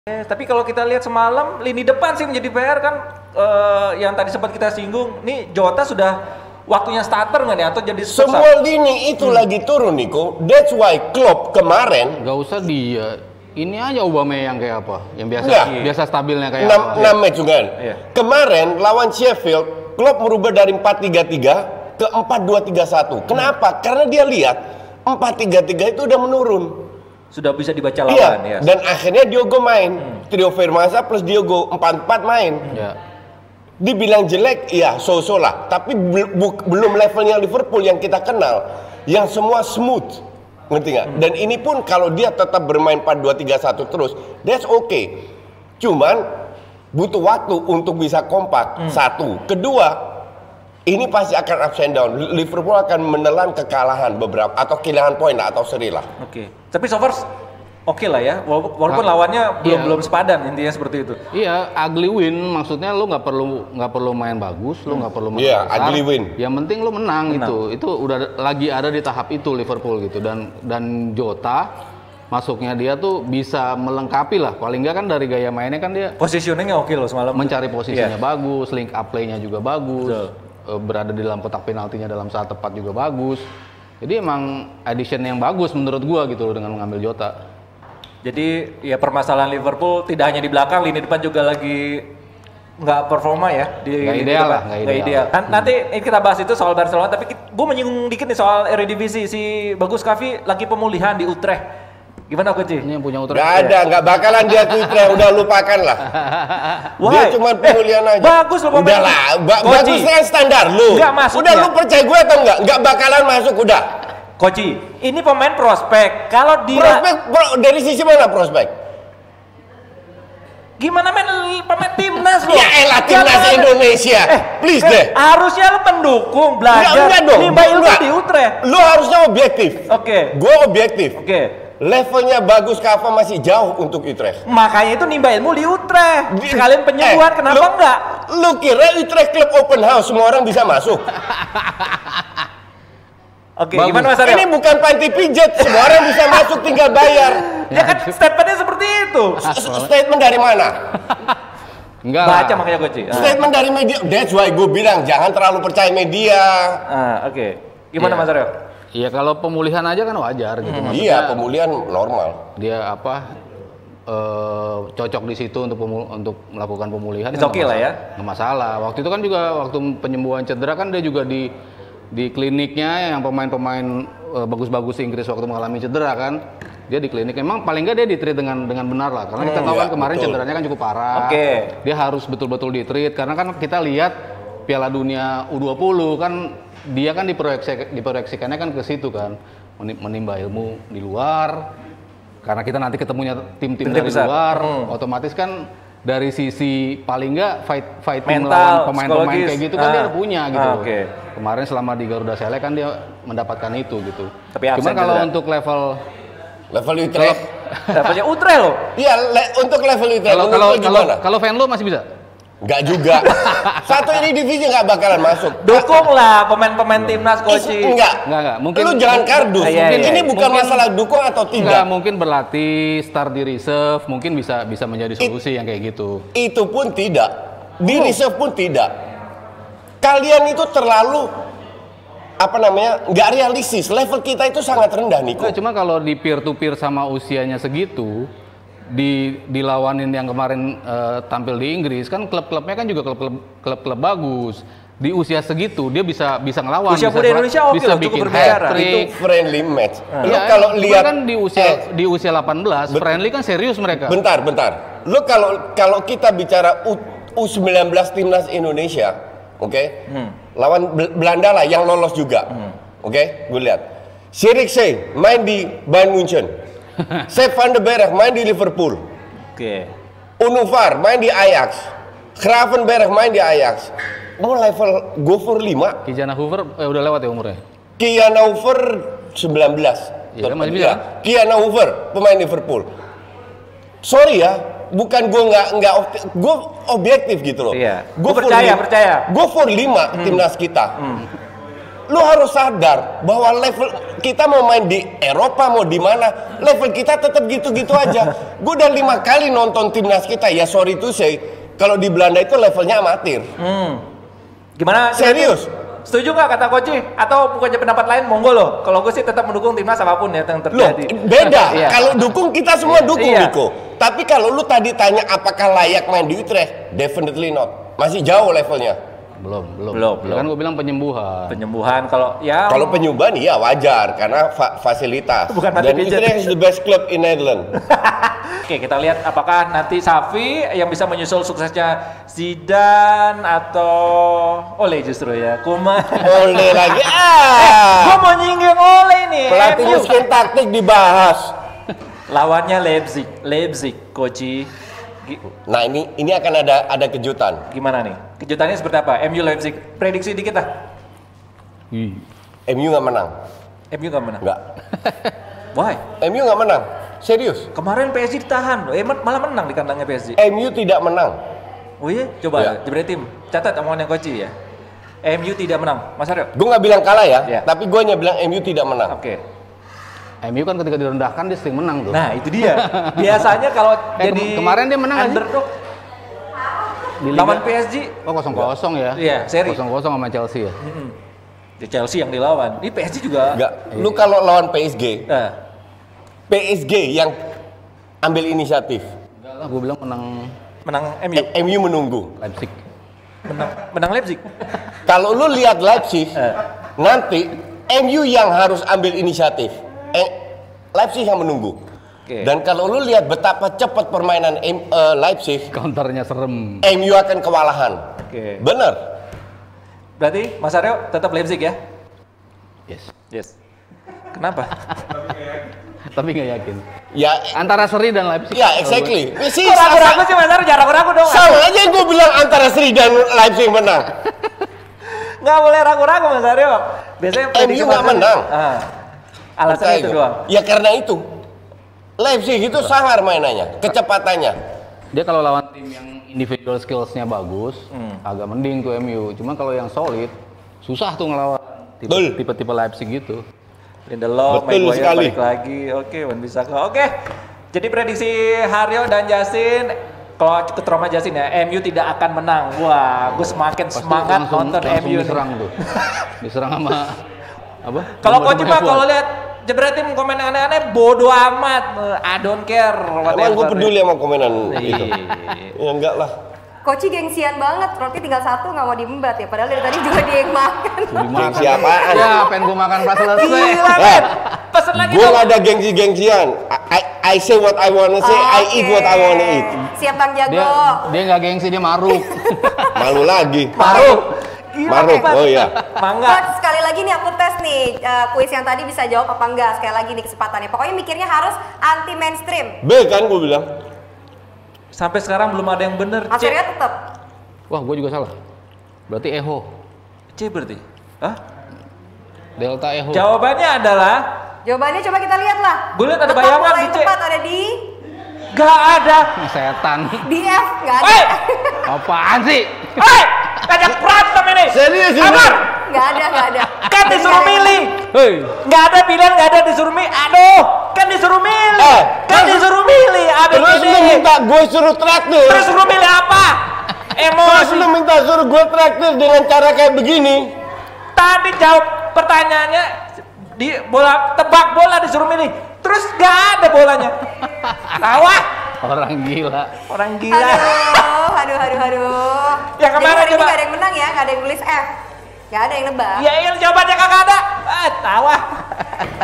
MU GAK BAKAL MENANG, BAGUS GAK AKAN MAIN DI UTRECHT
MU gak akan menang MU gak akan menang eh hasilnya menang 5-0 tapi kalo Bagus gak akan main di Utrecht akan kebalikannya gak ya kaya prediksi MU gak akan menang?